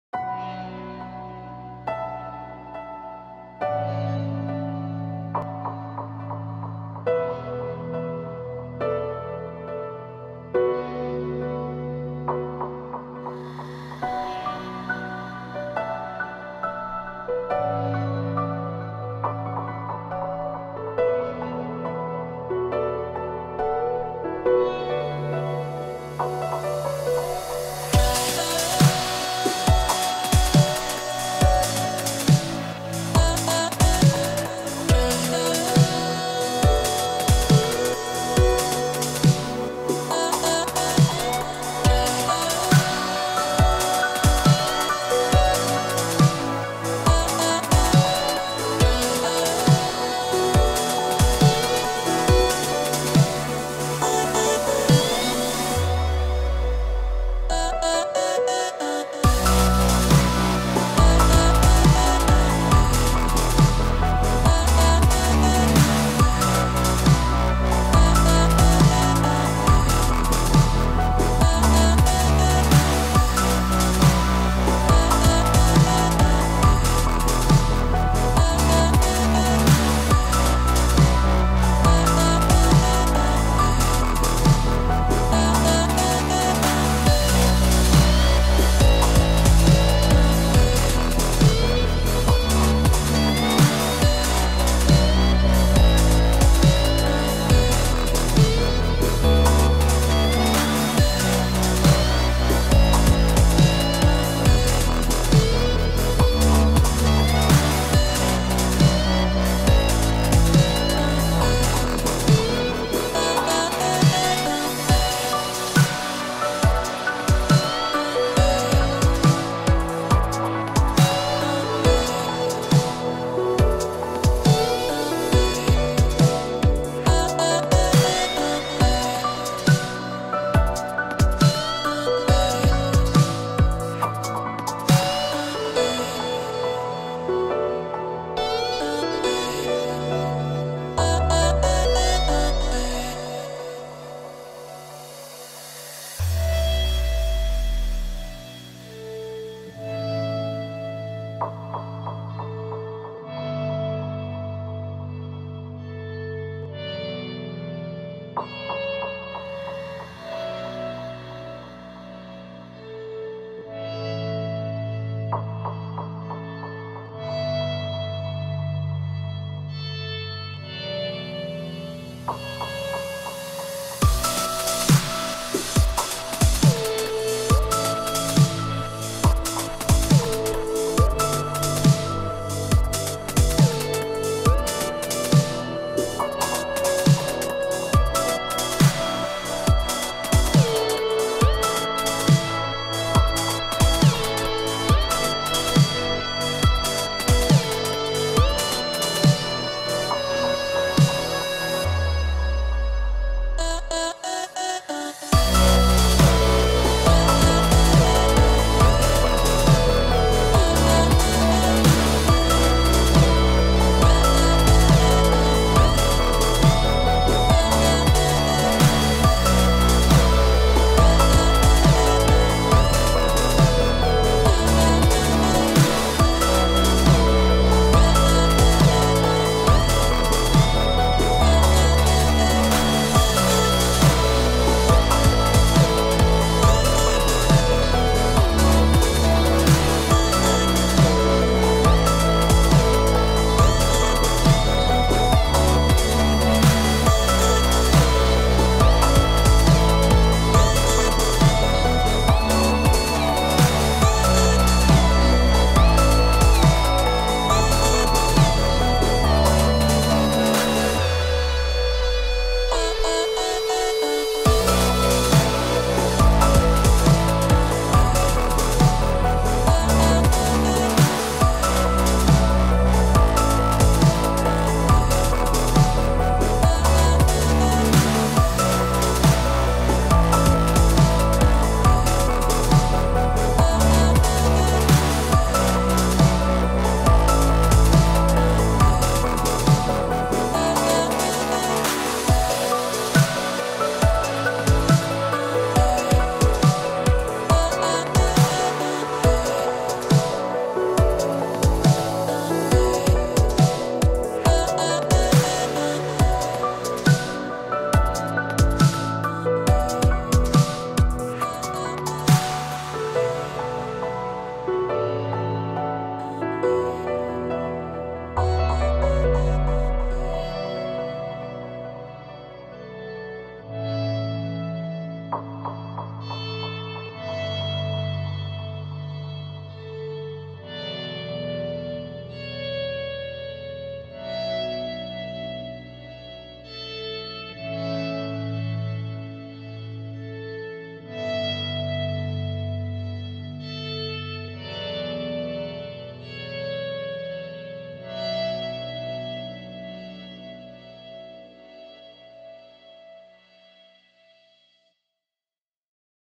Polson, Montana Take 2
Byron Leisek of ppgfun.com and I had a great flight on Jan 2nd Twenty Dirty-Deux in Polson, MT.  My first edit using Twixtor Basic, I'll work on it ;)